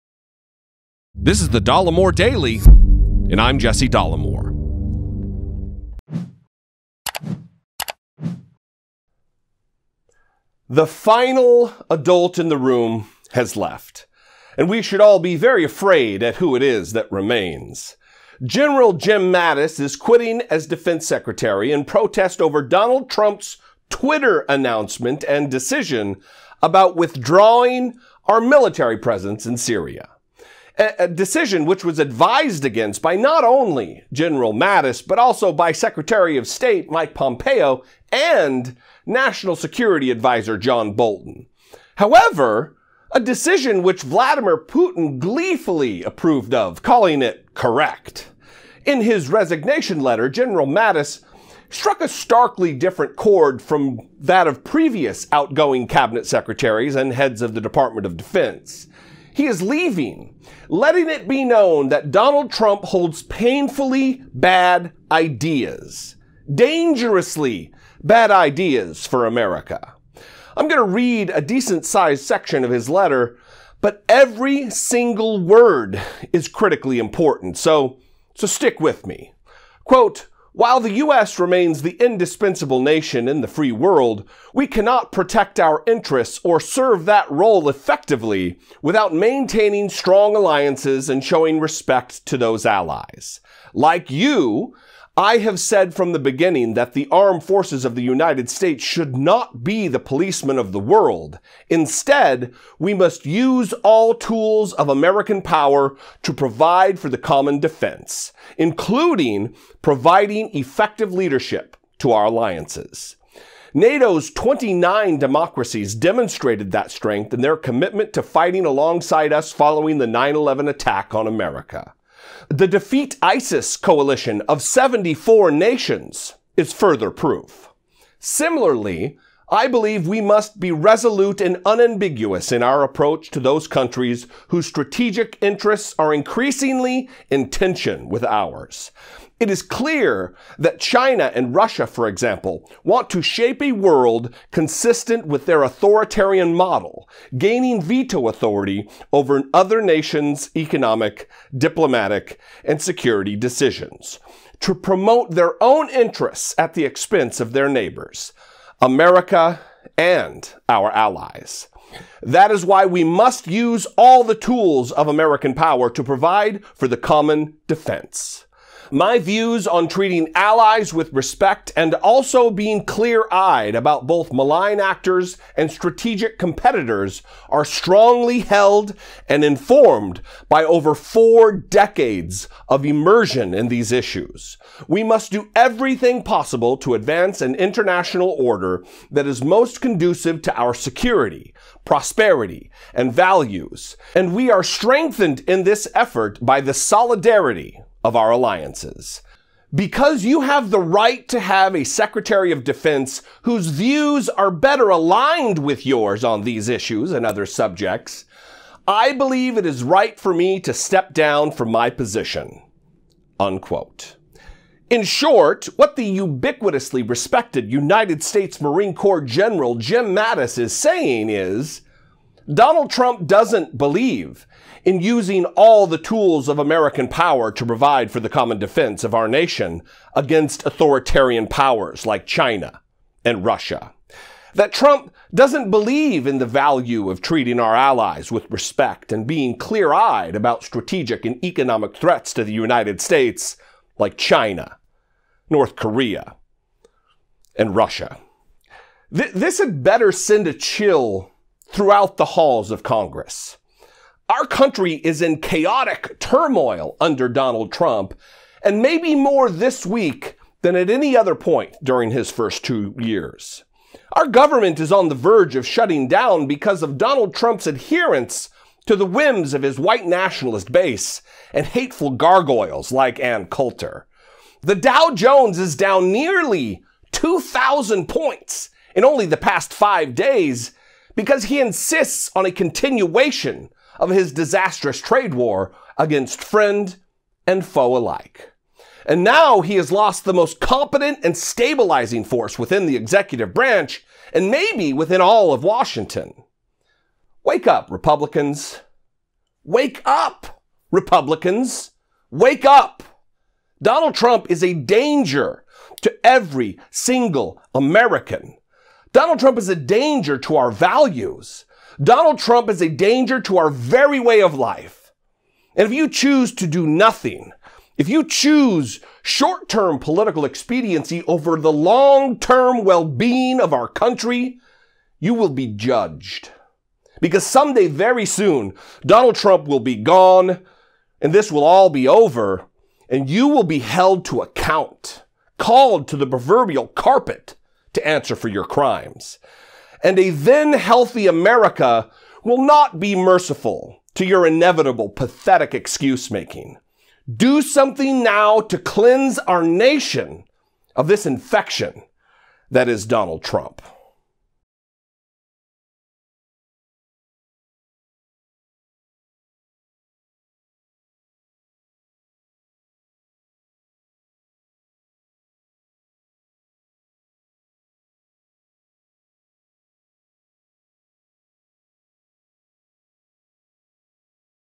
General Mattis Quits - Has Words for Dummy Trump - Republicans Better Wake Up!
Jesse talks about the surprise resignation of United States Marine Corps General - Secretary of Defense James "Mad Dog" Mattis, who wrote in his resignation letter that he could no longer remain in his post because of fundamental disagreements about protecting America and posture toward authoritarian regimes around the world.